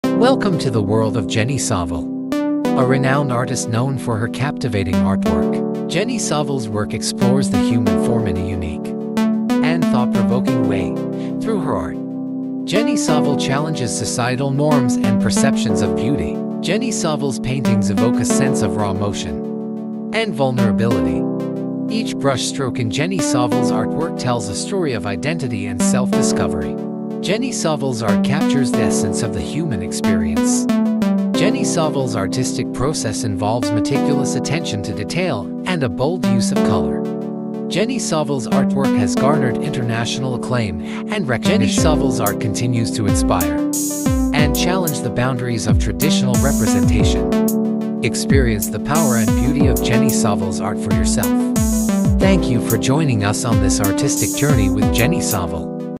Artist Jenny Saville
Hello art lovers! In this video, you will meet Jenny Saville, one of the leading figures of contemporary art. Jenny Saville is an original painter who expresses the complexity and beauty of the human body. His works prompt the viewers to deep thoughts by questioning the different forms and textures of the body. The artist's works bring innovation to the art world by presenting figurative art with a modern perspective. This video offers a look at Jenny Saville's artistic journey and impressive work. An unmissable content for art lovers!

» New Artist and their works will be uploaded! Turn on the '🔔' sign to be notified of new uploads!